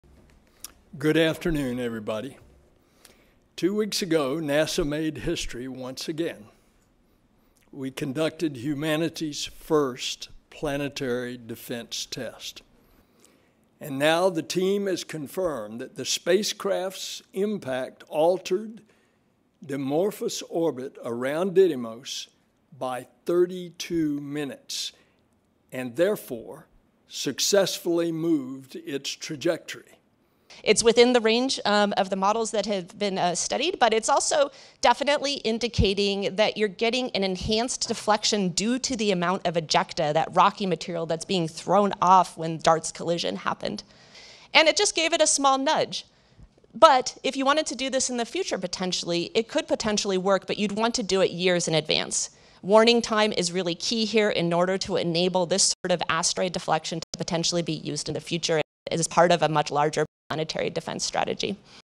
NASA confirms asteroid strike results in 'nudge'
NASA says the spacecraft that plowed into a small, harmless asteroid millions of miles away succeeded in shifting its orbit. (Oct. 11)